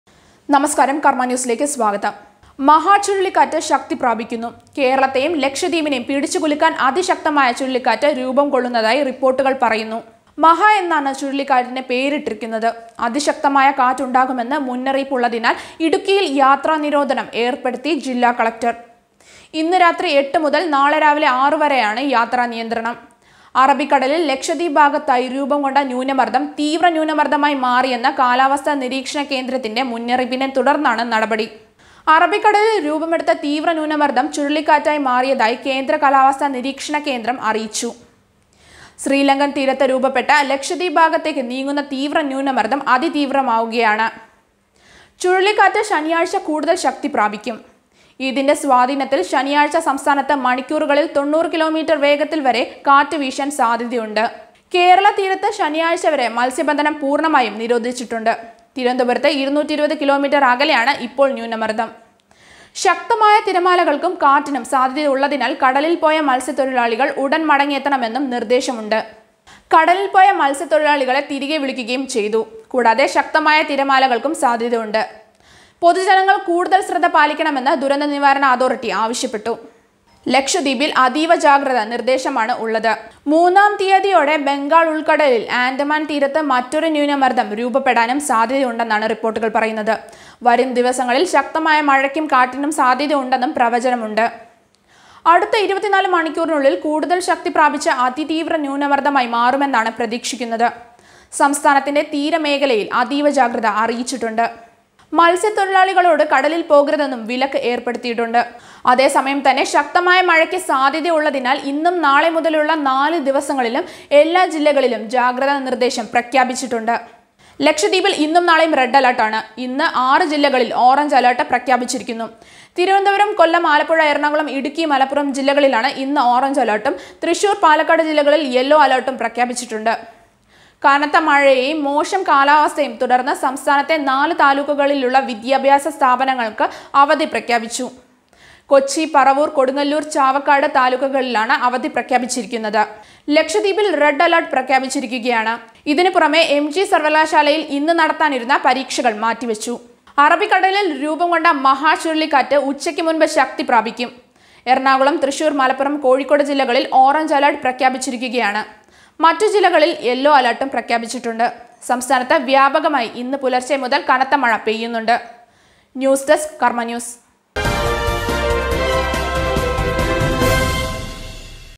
മഹാ' ചുഴലിക്കാറ്റ് ശക്തി പ്രാപിക്കുന്നു;വൻ ജാഗ്രത നിർദ്ദേശം ഇവിടങ്ങളിൽ യാത്ര ഒഴിവാക്കുക| karma news
'മഹാ' ശക്തി പ്രാപിക്കുന്നു . മൽസ്യ തൊഴിലാളികൾ കടലിൽ പോകരുത് ;യാത്രകൾ പരമാവധി  ഒഴിവാക്കുക .വൻ ജാഗ്രത നിർദേശം ഇടുക്കിയിൽ യാത്ര നിരോധനം ഏർപെടുത്തി .